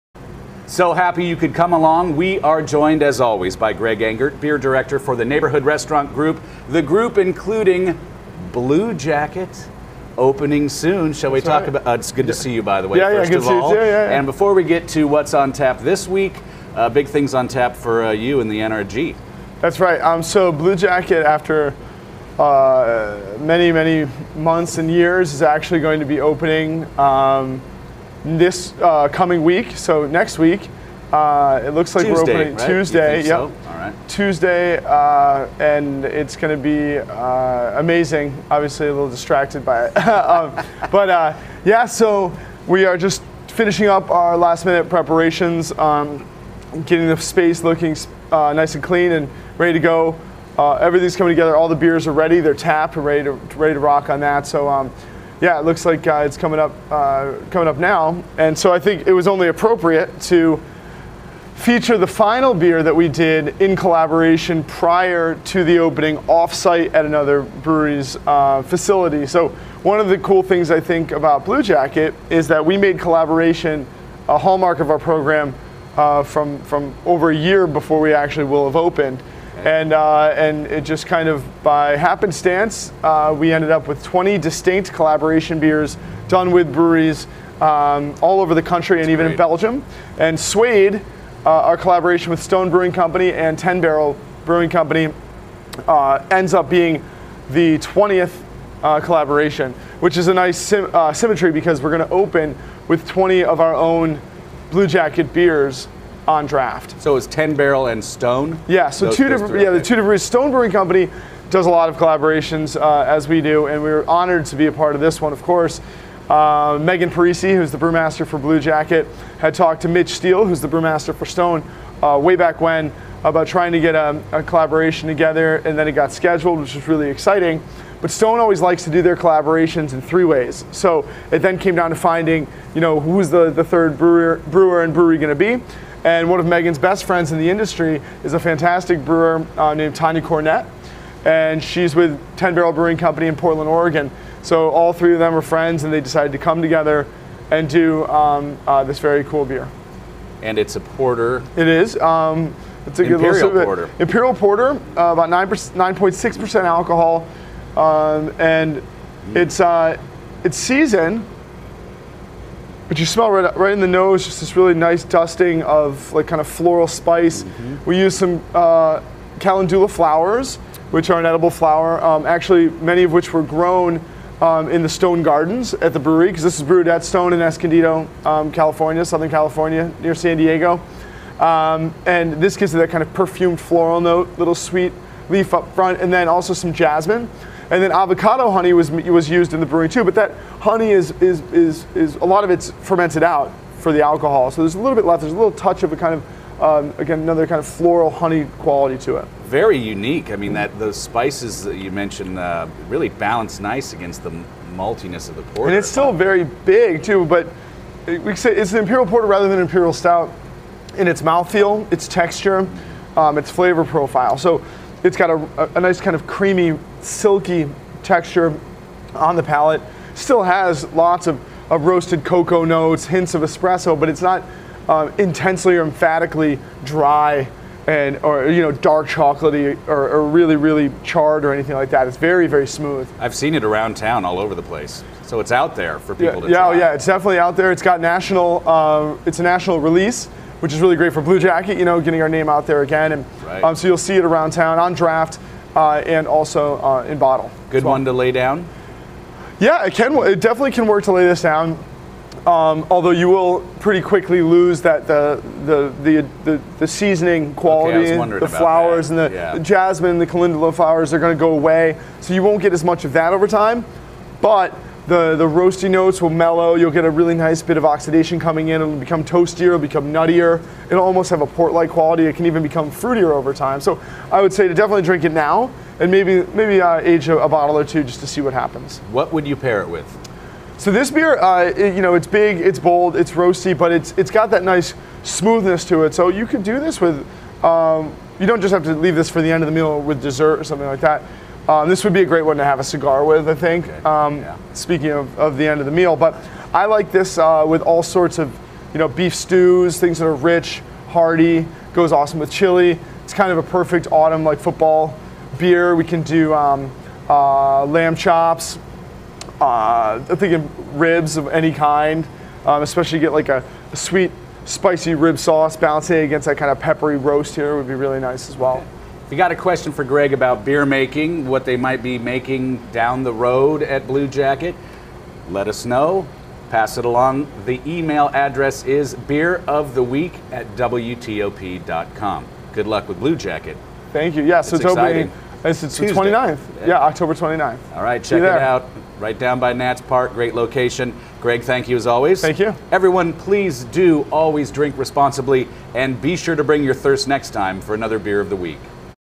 WTOP's Beer of the Week: 10 Barrel Stone Bluejacket Suede Imperial Porter
WASHINGTON -- It might look dark and forboding, but one sip could leave you thinking about blue skies and flowers.

WTOP's Brennan Haselton is joined by Neighborhood Restaurant Group Beer Director and James Beard Award nominee Greg Engert to talk about the 10 Barrel, Stone and Bluejacket collaboration, Suede Imperial Porter.